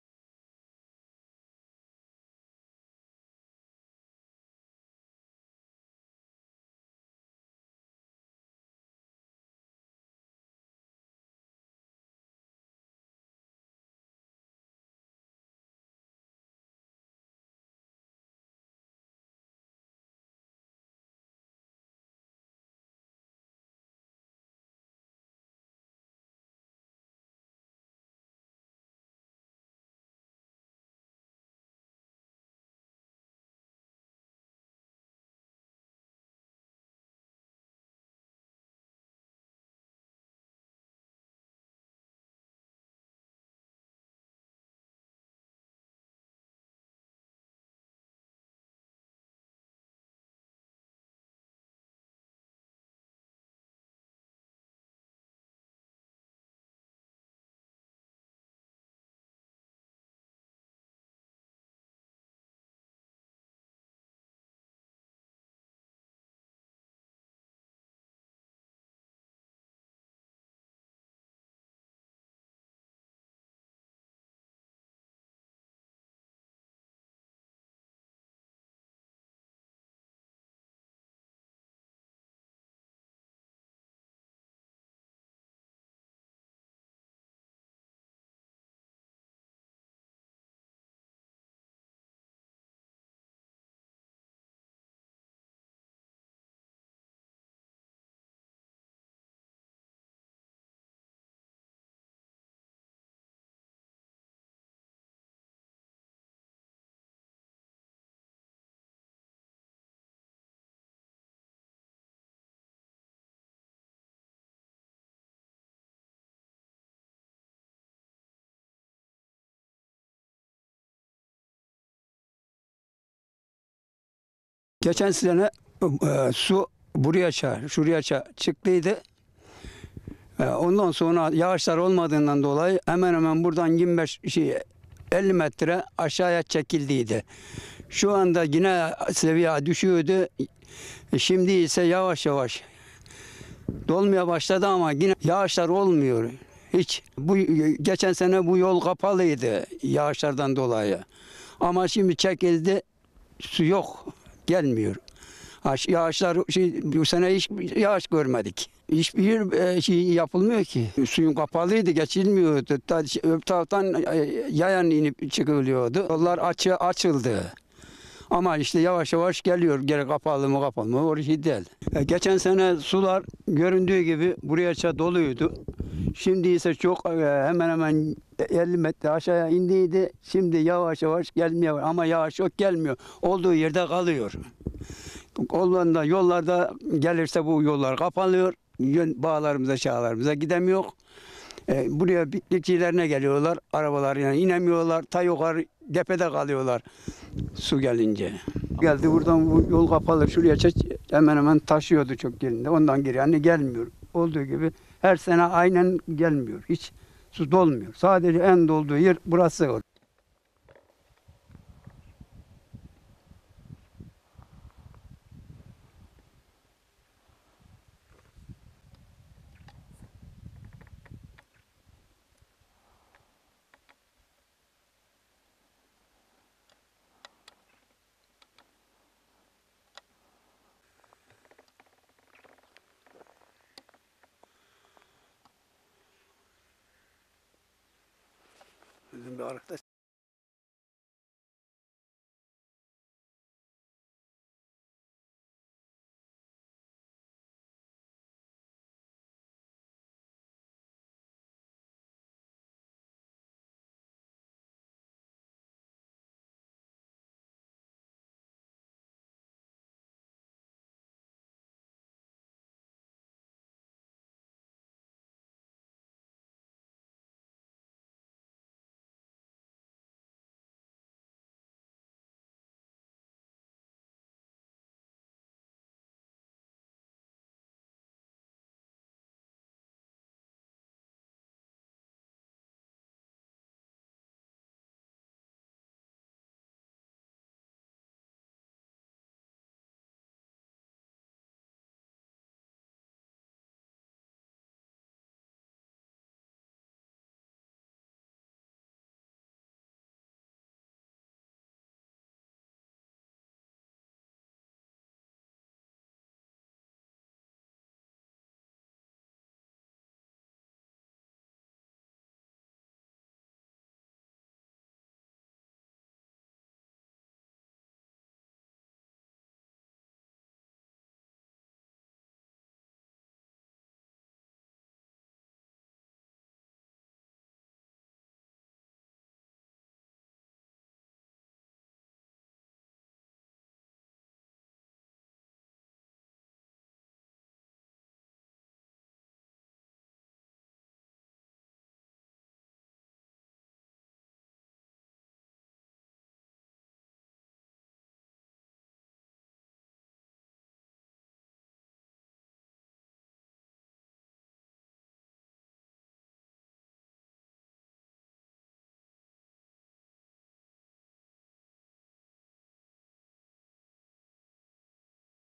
Kuraklık kapıda mı? Kayseri'de korkutan görüntü!
Yurt genelinde beklenen yağışların gerçekleşmemesi nedeniyle birçok noktada olduğu gibi Kayseri ilinde bulunan Yamula Barajı'nda da su seviyesi gözle görülür seviyede düştü. Detaylar da